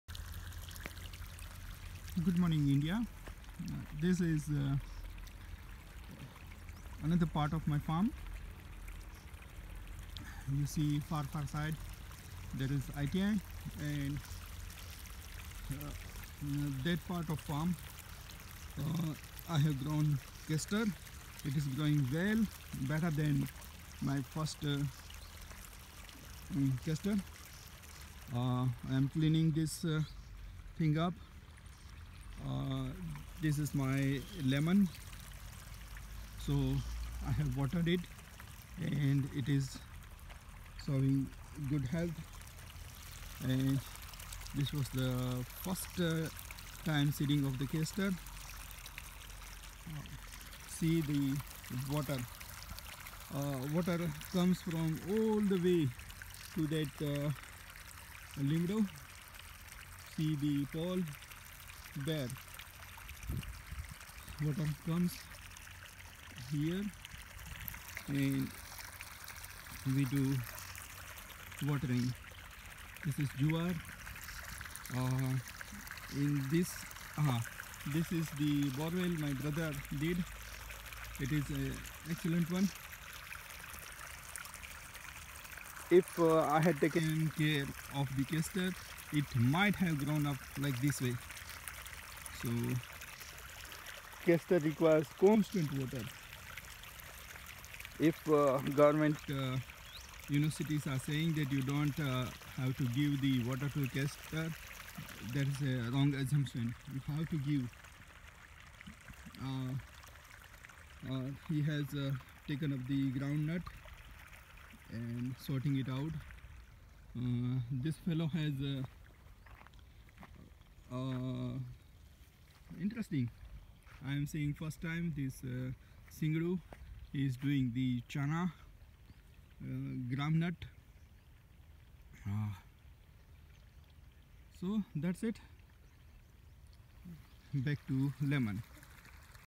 lemon plantations लिम्बु का खेत
360 view around the lemons. लिबु जुवार ओर पडोस के खेत  का विदीयो.
 5 लोगो के खेत से मेरी फेंसिग लगती है. मेरे पास लगभग 6 हेकटर जमिन हे जो बहुत ही बडा खेत होता  है. एक बार अगर मे चक्कर लगा लेता हु तो 1 किमि कि वोकिंग हो जाती है. 10 से 15 मिनिट लग जाता है. .